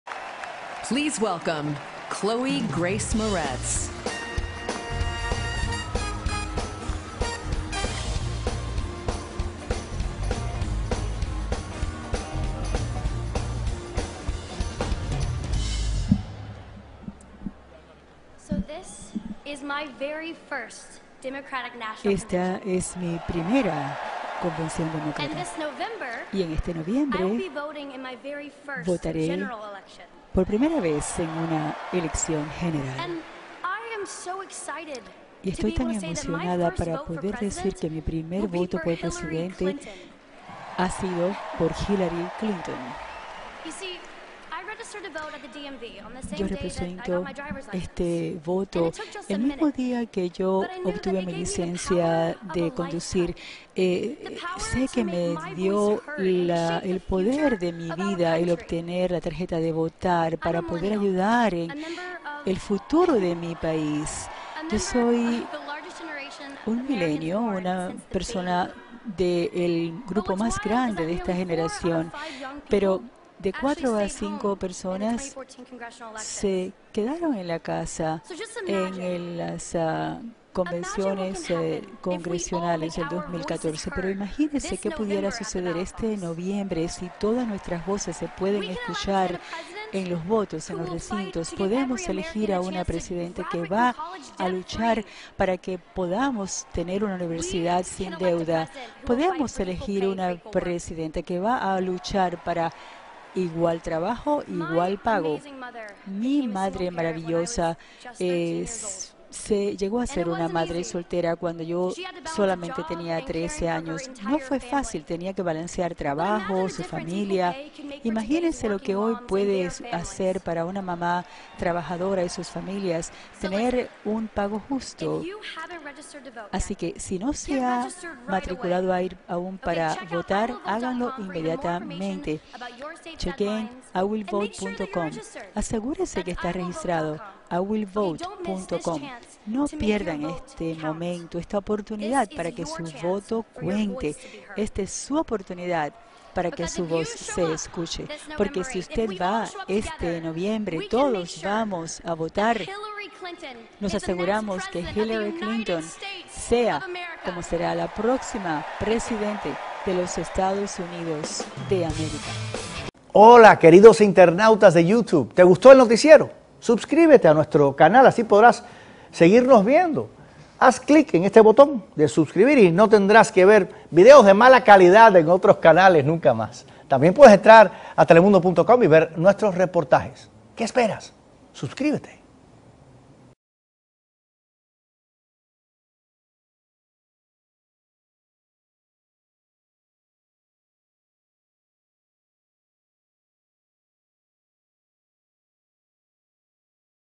Chloe Grave Moretz invita a los ‘millenials’ a votar | Noticias | Noticias Telemundo
Video oficial de Noticias Telemundo. La joven actriz pidió a los chicos de su generación que salgan a votar para ayudar a formar un buen futuro para el país.